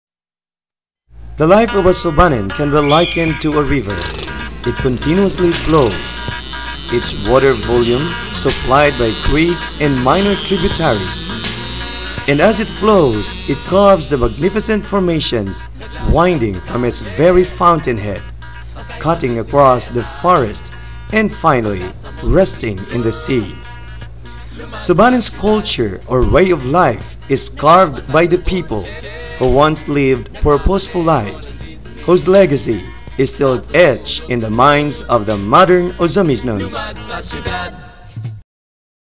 Subanen 2
A cultural show featuring the Subanen  during the 60th charter celebration.